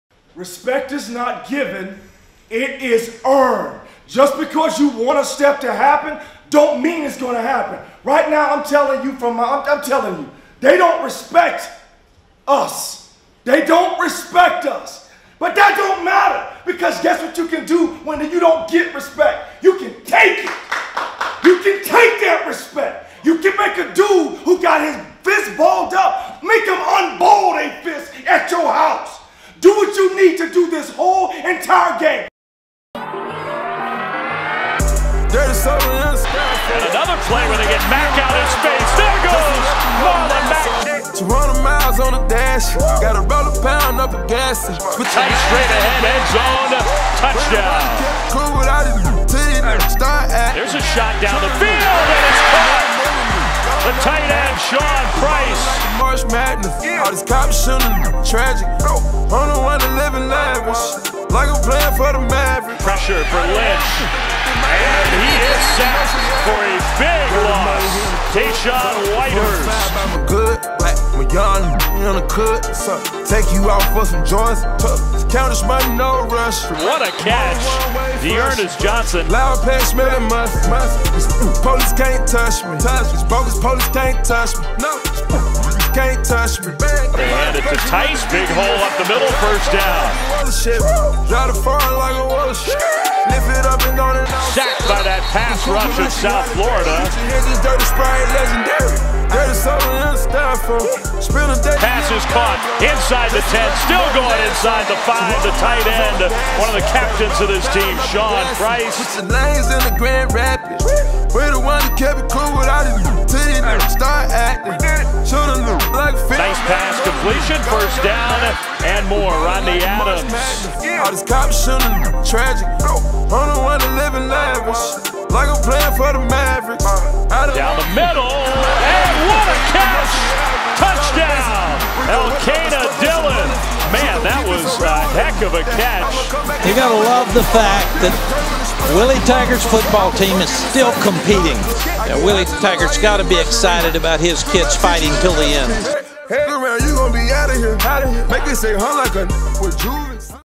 2015 USF Football - Earn Their Respect
Highlight video shown to the 2015 USF Football team before they took on Syracuse on October 10th, 2015.